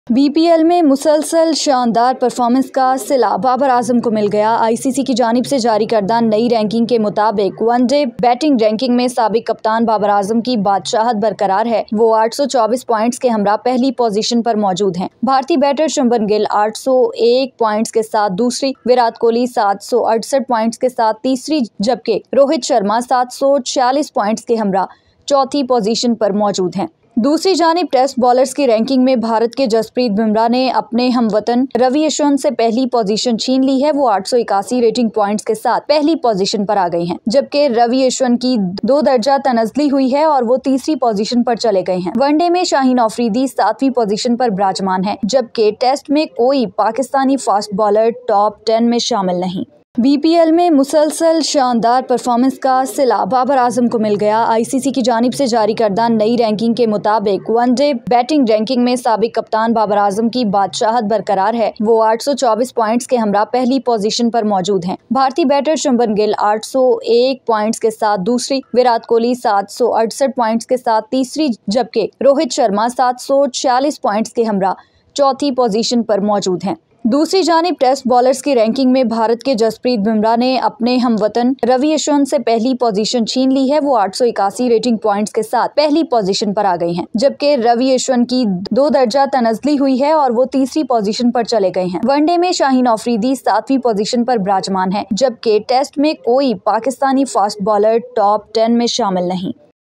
Good News For Baber Azam | Latest ICC ODI rankings | Baber Azam No 1 ODI batter | 7NewsHD
Pakistan's Babar Azam maintains his place at the top of the ODI batter rankings in front of India trio Shubman Gill (second), Virat Kohli (third) and Rohit Sharma (fourth), while Abbott jumps 52 places to equal 18th on the list for all-rounders after also contributing 69 runs from one innings with the bat against the West Indies.